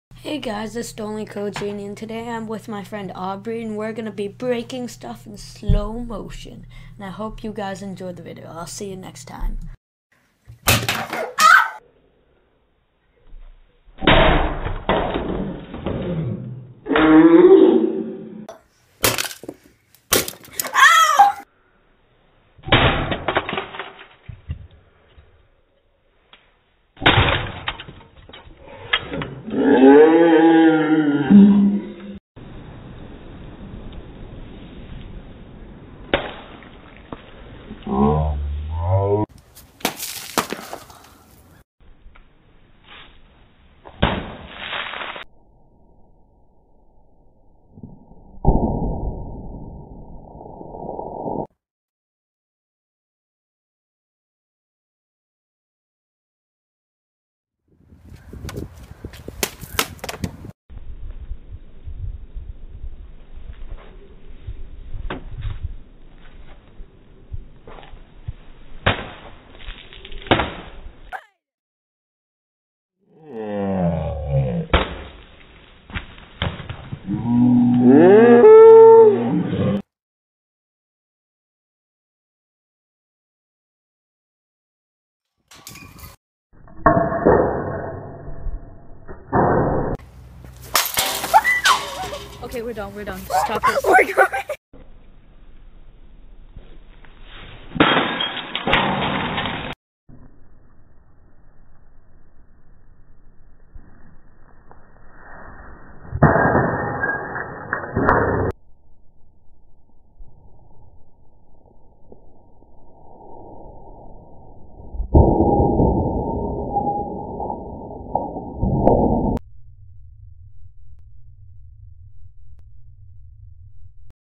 Slow Motion Breaking - Soda cans and other stuff
Cole and Special Guest Aubrey break some stuff in sloooww mooootion